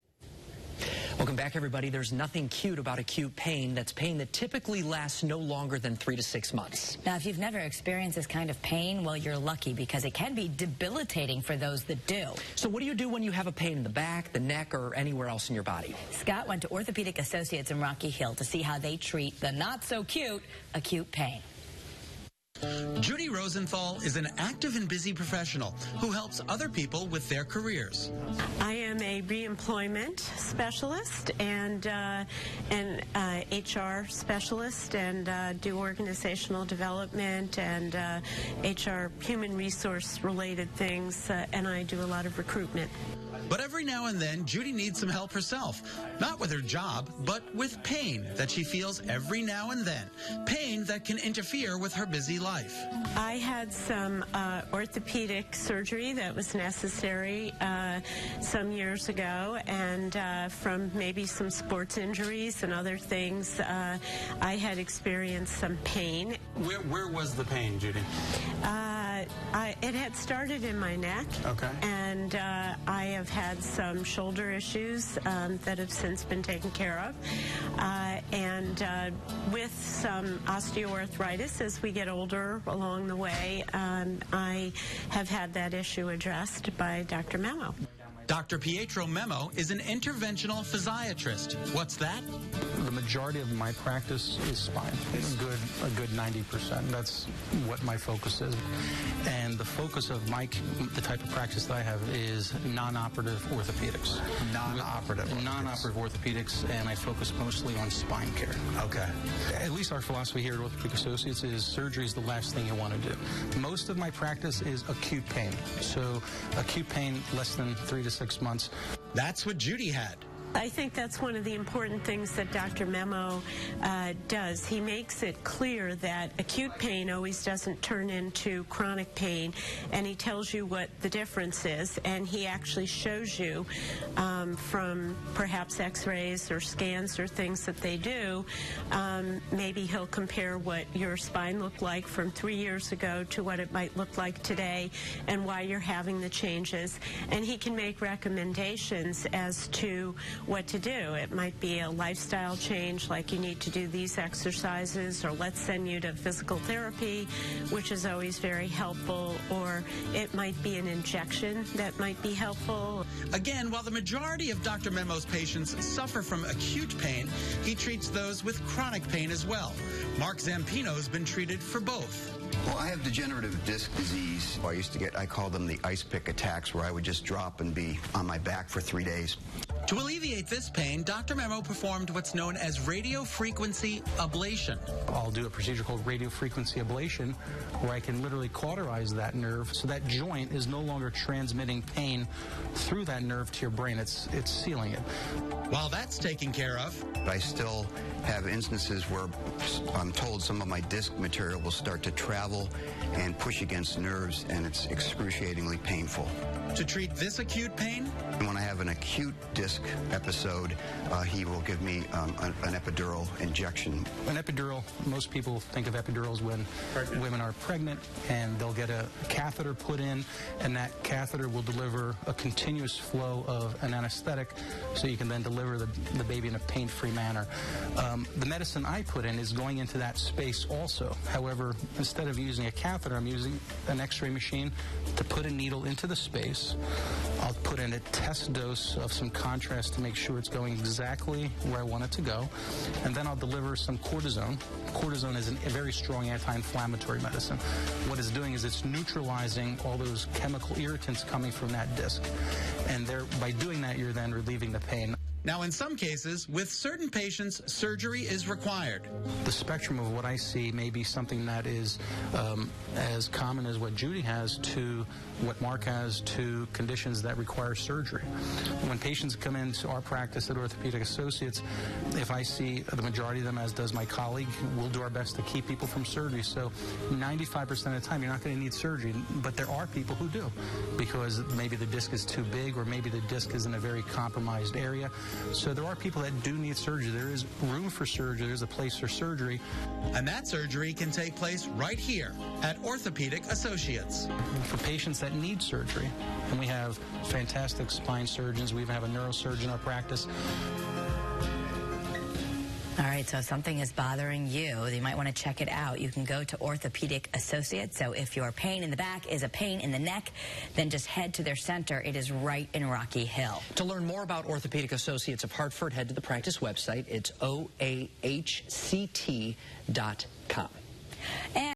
Dr. Pietro Memmo talks about pain relief
Dr. Pietro Memmo talks with Better CT about pain relief options for acute or chronic pain.

Connecticut's Largest and Most Respected Orthopedic Specialty Group